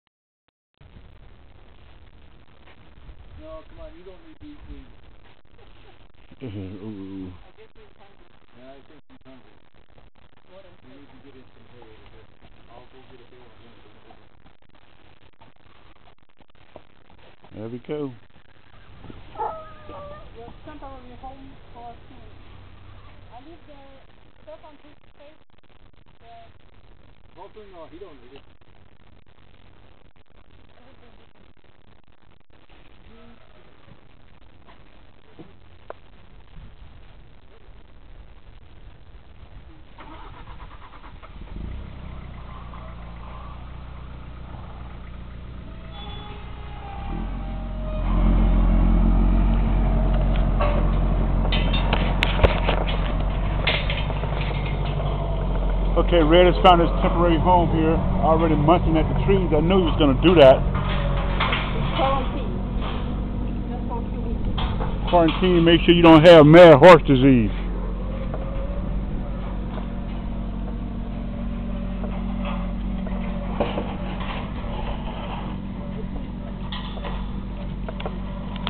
Red's painful walk
I hope to post in a few months a video of him walking without pain or maybe even running!!